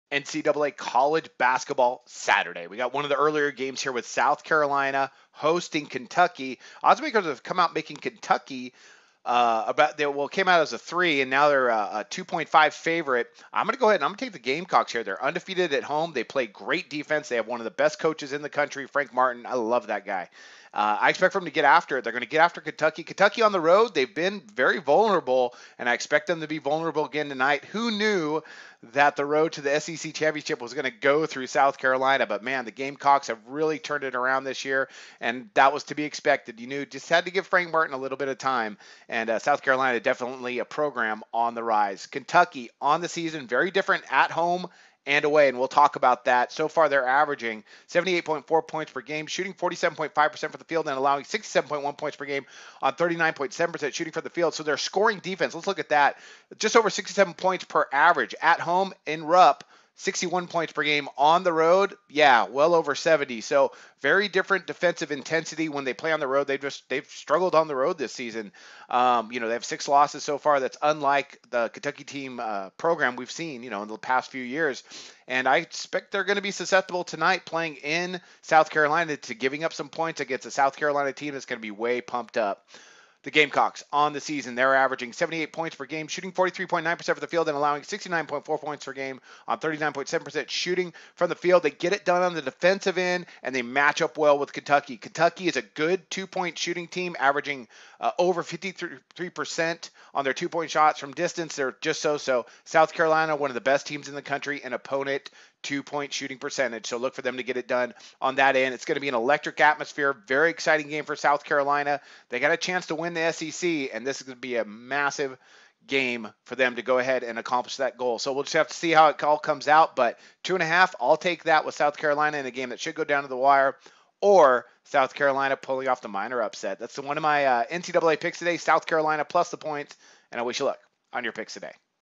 Prime Betting: Kentucky vs. South Carolina Free Picks
Kentucky has had its share of growing pains this season and today they travel to a much improved South Carolina team. NCAA basketball odds makers have come out making the Wildcats a small road favorite much to my surprise. Head coach of the Gamecocks Frank Martin has built something solid at South Carolina and today will be a major test. Has Kentucky turned the corner to win this game on the road, or will South Carolina keep surprising people?